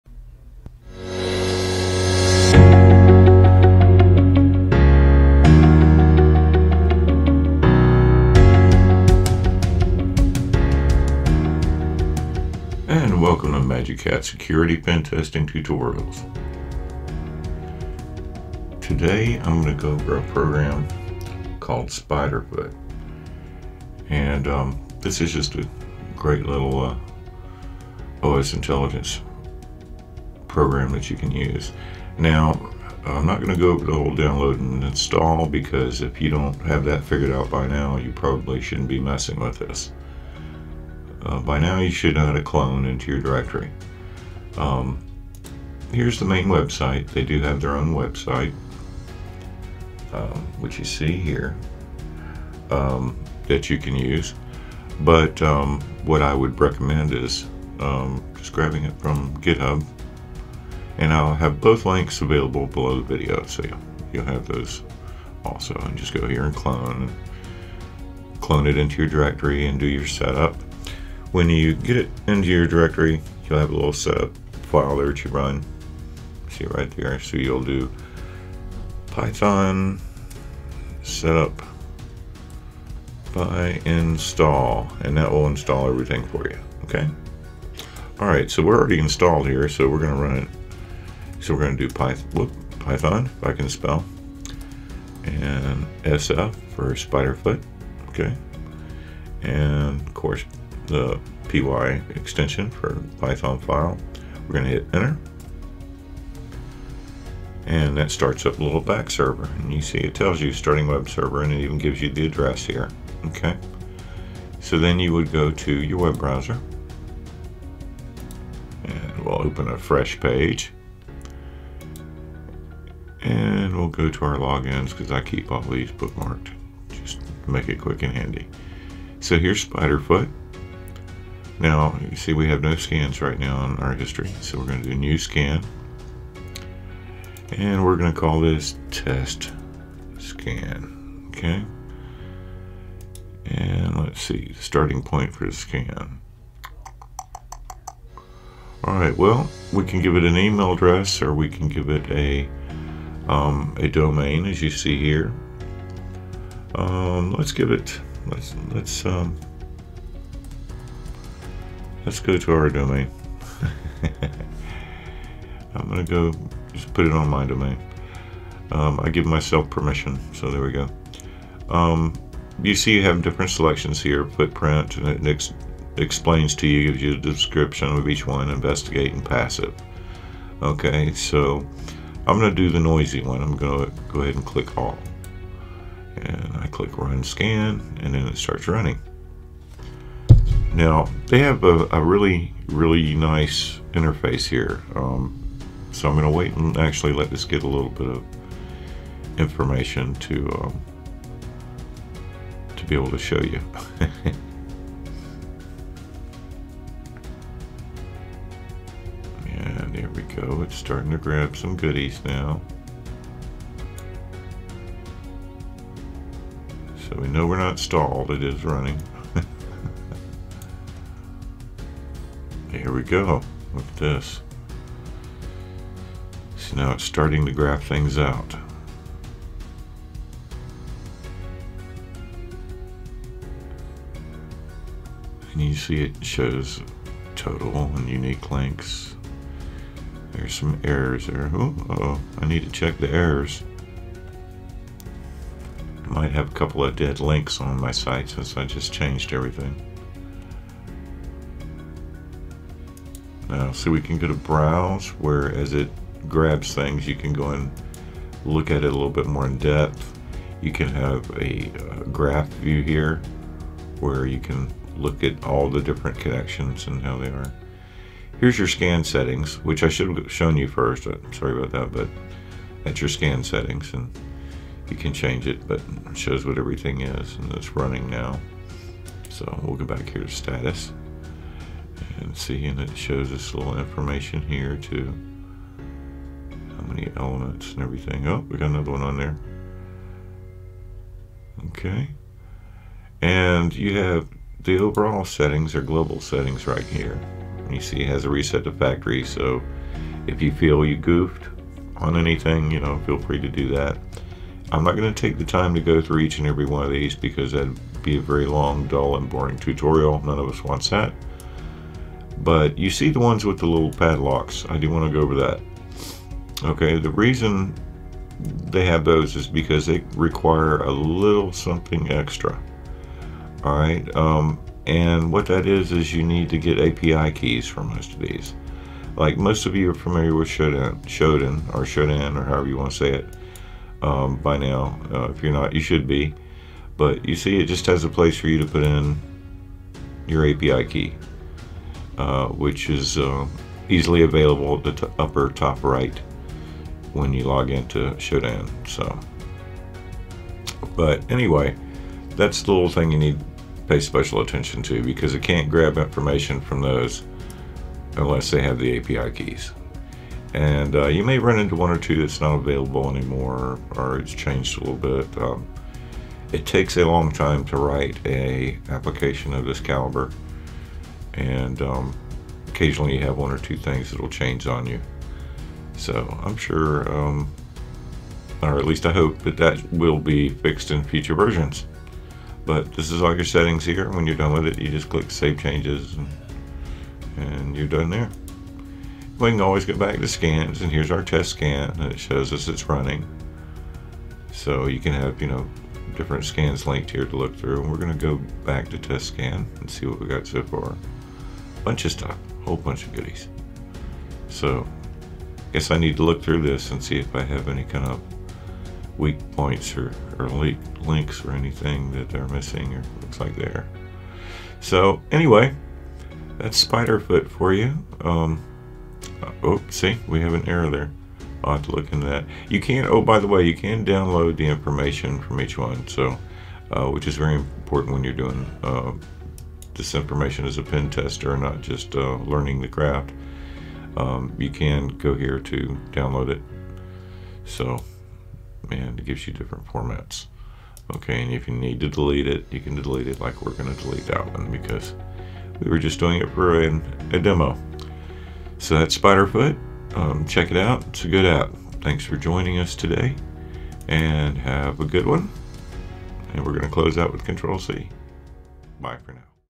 Spiderfoot OSINT Tutorial - Parrot Security OS
This is a brief tutorial/overview of the Spiderfoot OSINT for pentesting purposes. This is a great little pentesting tool for cyber security professionals and is completely open source.

(This is a subscription based web run service you can access anywhere at anytime. Great for pentesters and IT departments.)